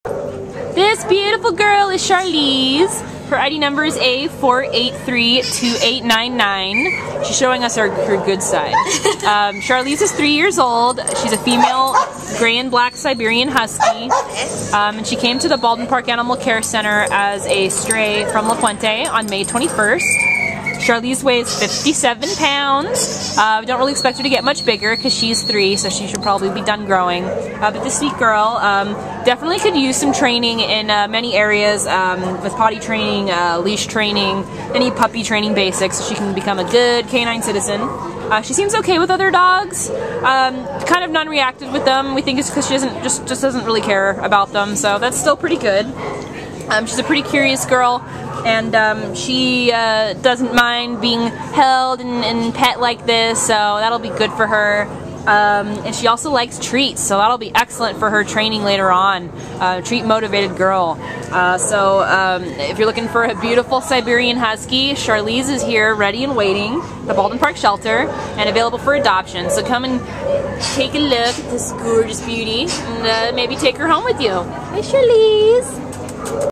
A4832899 Charlize | Siberian Husky Mix
A4832899 Charlize is a snazzy 3-year-old gray-and-black female Siberian Husky mix who came to the Baldwin Park Animal Care Center as a stray from La Puente on May 21st. Weighing 57 lbs, Charlize has a medium energy and is sure to settle into a stylish, knockout beauty once she gets the care and love she deserves (she seems to have been kept in a yard and has fly-strike marks on her ears to prove it). Charlize will also benefit from some leash training to correct her pulling and to teach her to stride like the good canine citizen and athlete we know she can be. She showed us that she’s okay with other dogs—nonreactive and, in truth, not very interested. She is curious about her surroundings and people, however, and she’s big on treats, so we assume any training will go easily. We think the best future for her would be as an indoor pet with an individual or family that understands the Husky breed and wants to provide Charlize with the love, care, and activities she needs to be one movie-star pup!